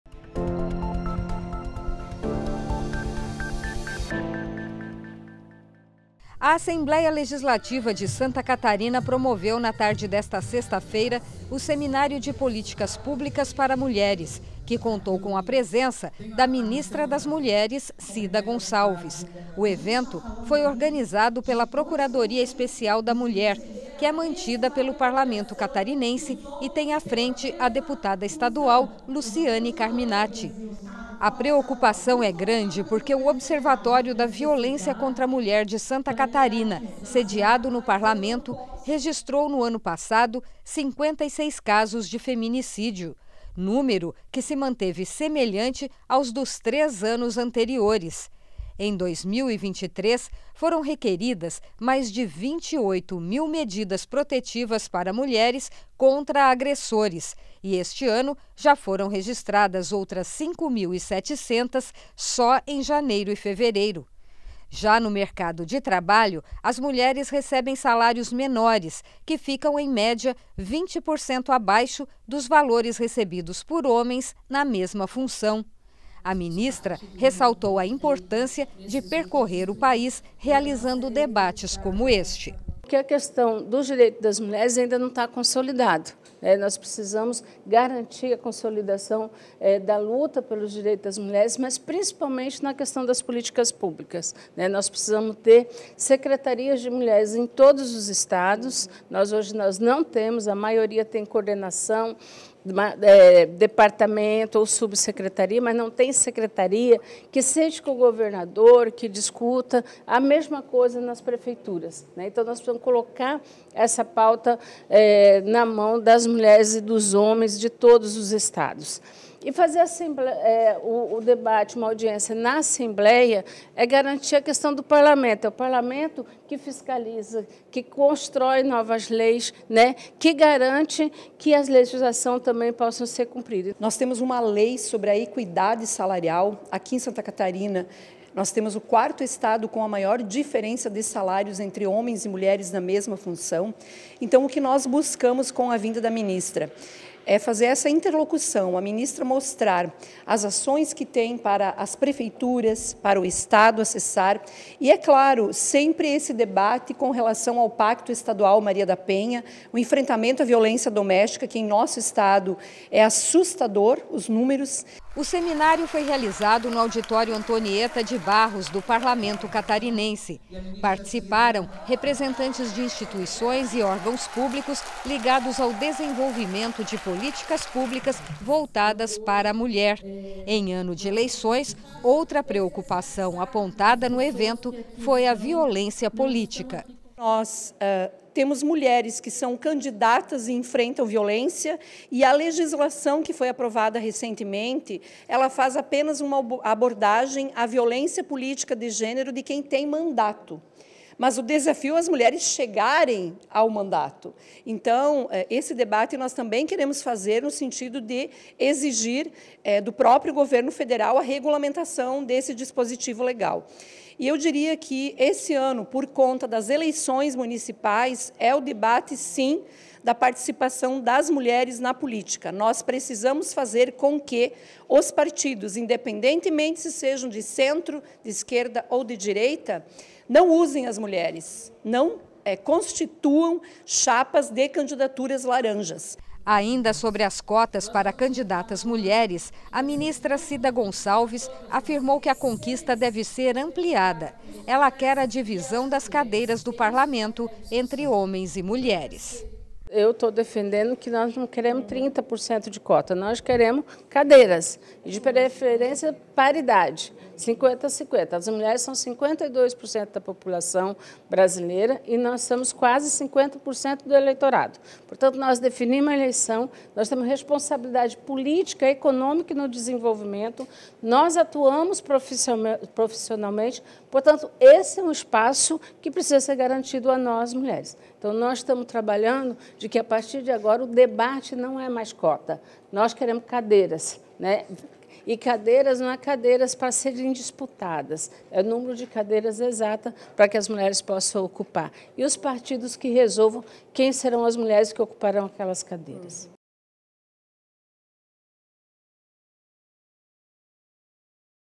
Ministra das Mulheres participa de seminário na Alesc - 12/04/24
A Ministra das Mulheres Cida Gonçalves participou na tarde desta sexta-feira, na Assembleia Legislativa de Santa Catarina,  do Seminário de Políticas Públicas para Mulheres. O evento, realizado no Auditório Antonieta de Barros, foi uma iniciativa da Procuradoria Especial da Mulher mantida pelo parlamento catarinense e que tem à frente, a deputada estadual Luciane Carminatti (PT). Na pauta, ações para conter os índices de feminicídio e de violência contra a mulher, programas de atendimento a mulheres em situação de violência, isonomia salarial entre homens e mulheres e a violência política contra a mulher.